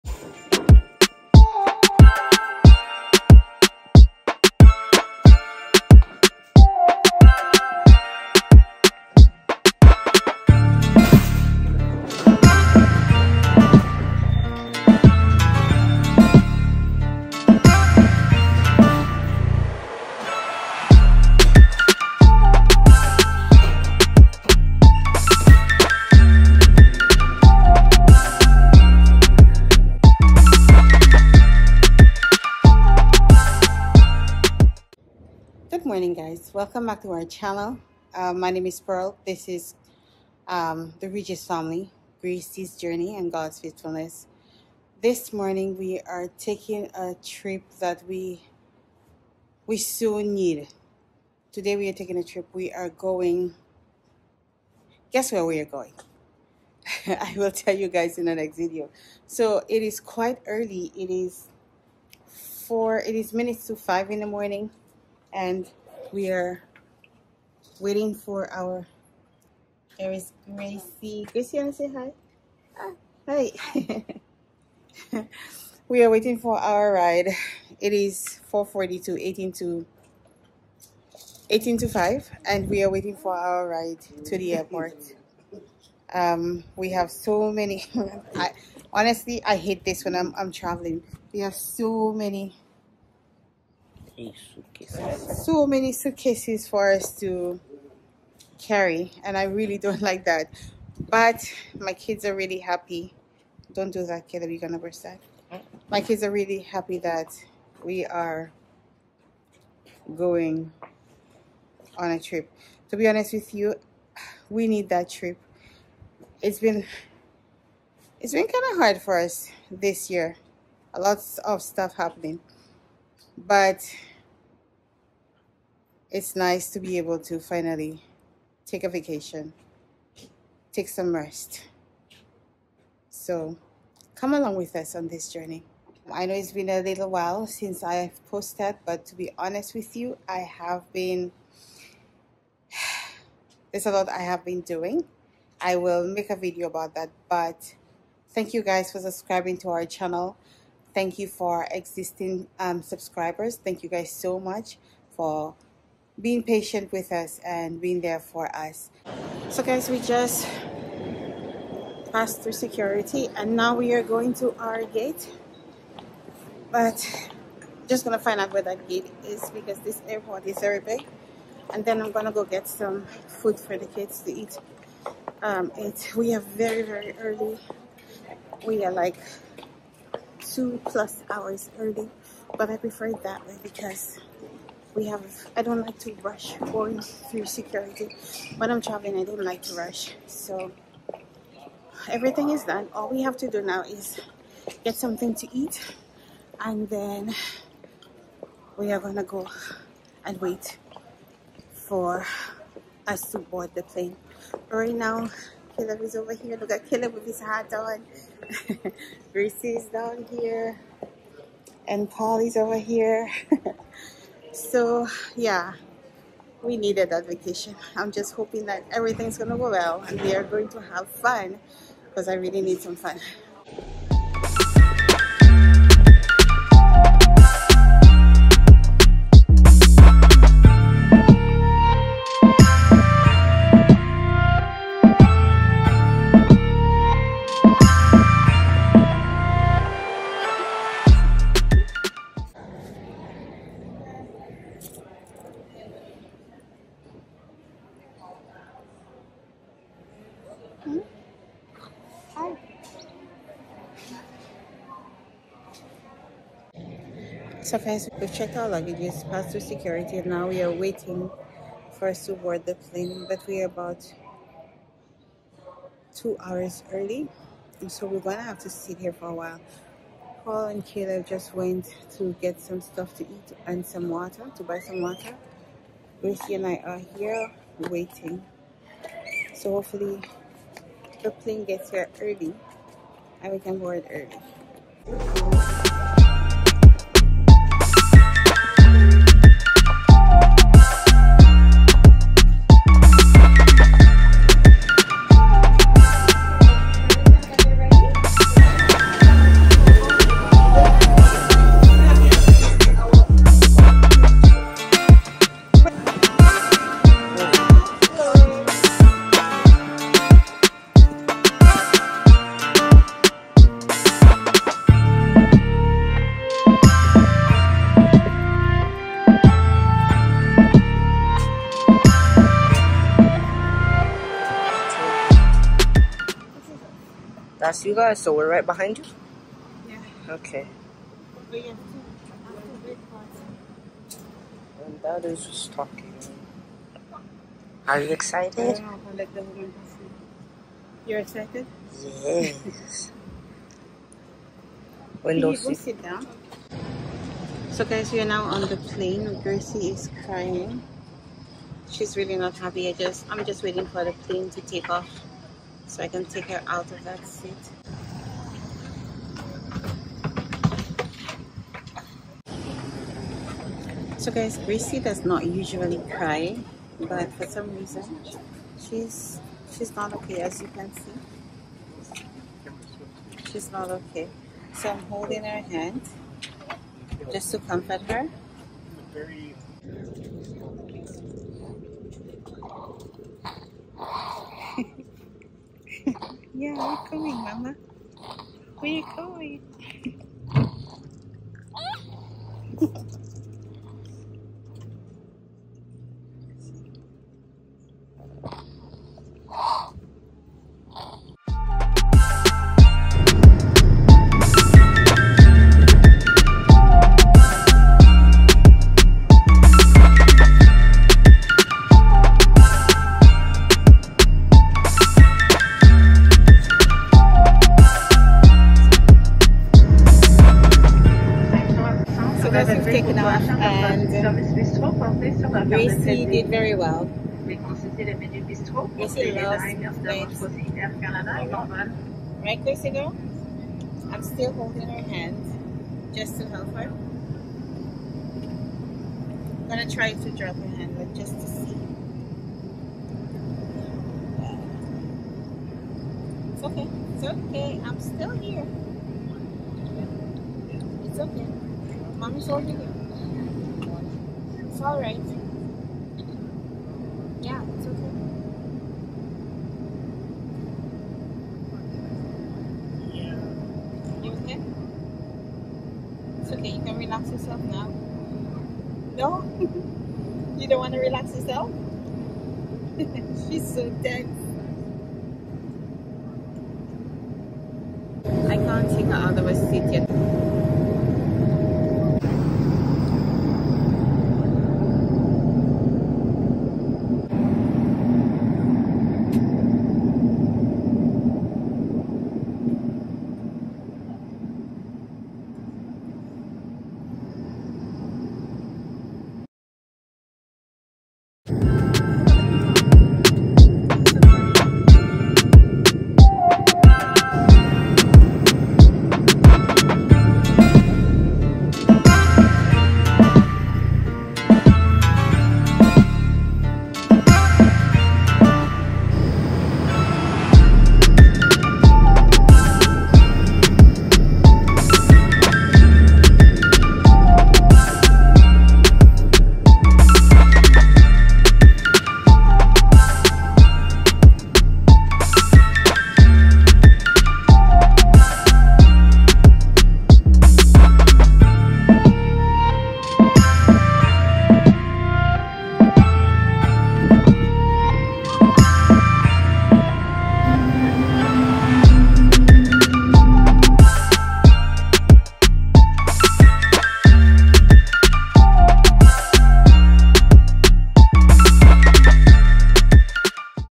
Travelling To My Country For The First Time After 20 Years
This Video is the beginning of our trip to St. Lucia